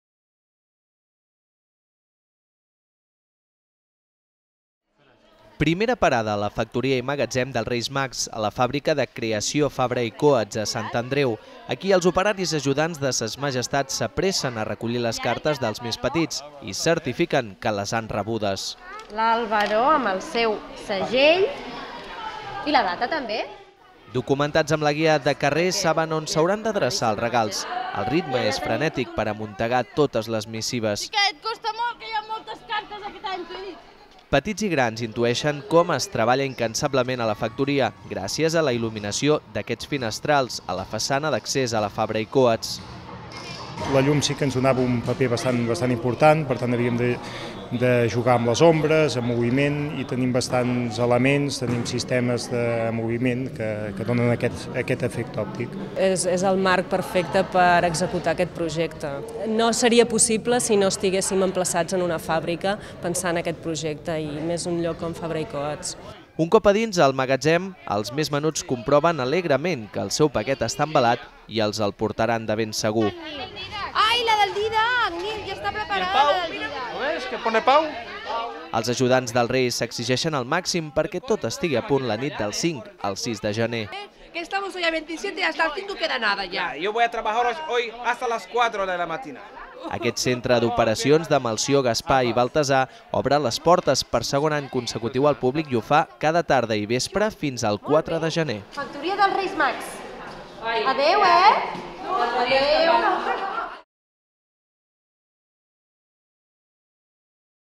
Descobriu com els Reis distribuiran els regals!
Fins el 4 de gener, la Fàbrica de Creació Fabra i Coats, a Sant Andreu, és el centre d'operacions dels Reis Mags, des d'on es distribuiran els regals arreu de la ciutat.